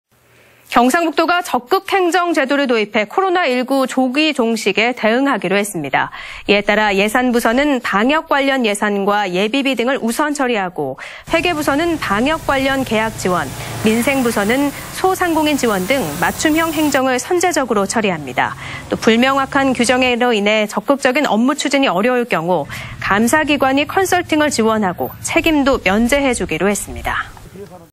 경북도, '적극 행정' 도입 코로나 선제 대응 / 안동MBC
2020/03/05 14:44:27    작성자 : 김건엽

경상북도가 '적극 행정' 제도를 도입해
코로나19 조기 종식에 대응하기로 했습니다.
◀ＥＮＤ▶

이에 따라 예산부서는 방역관련 예산과
예비비 등을 우선 처리하고
회계부서는 방역관련 계약지원,
민생부서는 소상공인 지원 등
맞춤형 행정을 선제적으로 처리합니다.

또 불명확한 규정으로 인해
적극적인 업무추진이 어려울 경우
감사기관이 컨설팅을 지원하고
책임도 면제해 주기로 했습니다.